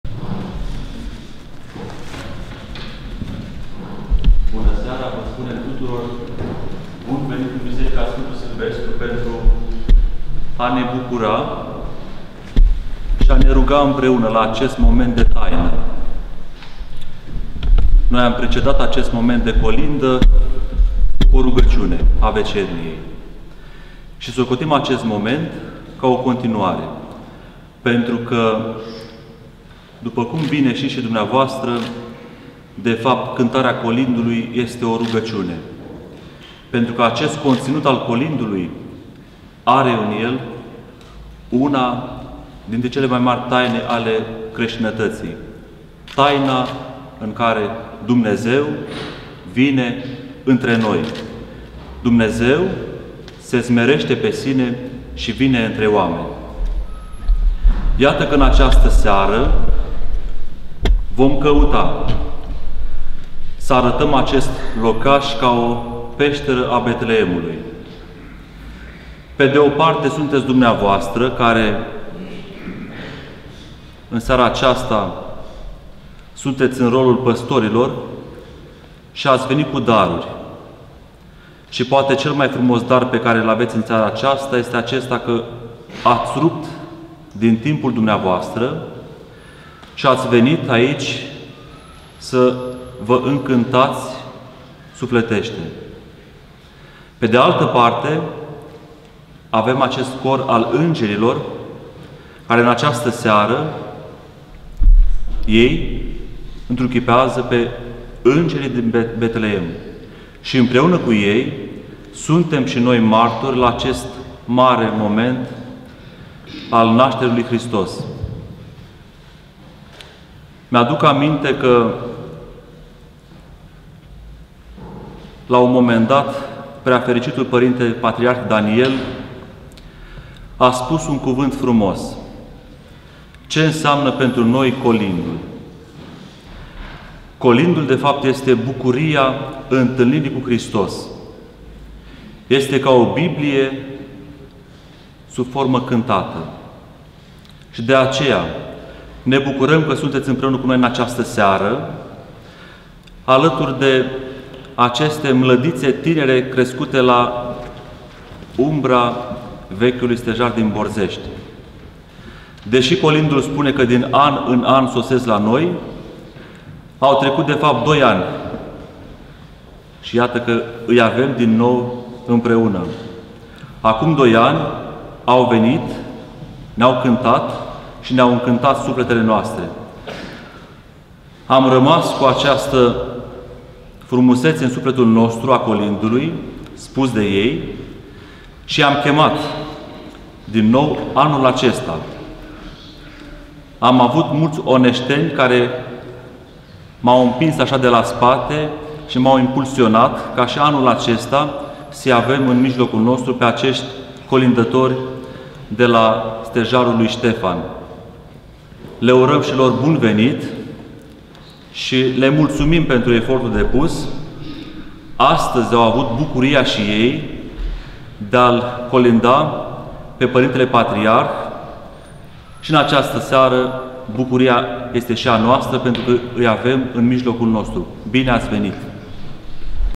(01) Pr. Adrian Argintariu - Introducere - Concert de Colinde tradiționale românești (2019.12.19)
Joi, 19 decembrie 2019, începând cu ora 19:00, a avut loc la biserica „Sfântul Silvestru” din București concertul de Colinde tradiționale românești intrepretat de corul psaltic Filomelos.

Corul psaltic Filomelos activează sub binecuvântarea Înaltpreasfințitului Părinte Ioachim, Arhiepiscop al Romanului și Bacăului, iar din anul 2013 dă răspunsurile la strana Bisericii Sfântului Ștefan cel Mare din Borzești (Onești, județul Bacău). Preot paroh: Petrică Cautis.

Gazdă: Pr. Adrian Argintariu.